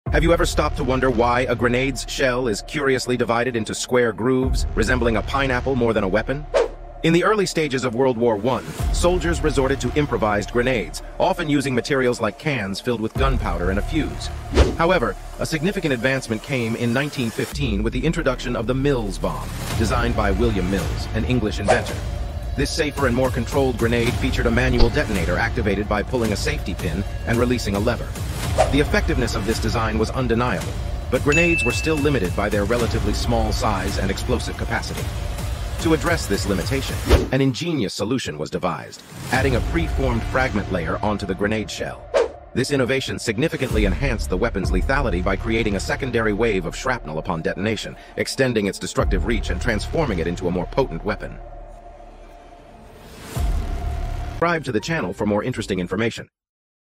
The Science Behind the Grenade's Grooves
Have you ever stopped to wonder why a grenade's shell is curiously divided into square grooves, resembling a pineapple more than a weapon? In the early stages of World War I, soldiers resorted to improvised grenades, often using materials like cans filled with gunpowder and a fuse. However, a significant advancement came in 1915 with the introduction of the Mills bomb, designed by William Mills, an English inventor. This safer and more controlled grenade featured a manual detonator activated by pulling a safety pin and releasing a lever. The effectiveness of this design was undeniable, but grenades were still limited by their relatively small size and explosive capacity. To address this limitation, an ingenious solution was devised – adding a pre-formed fragment layer onto the grenade shell. This innovation significantly enhanced the weapon's lethality by creating a secondary wave of shrapnel upon detonation, extending its destructive reach and transforming it into a more potent weapon.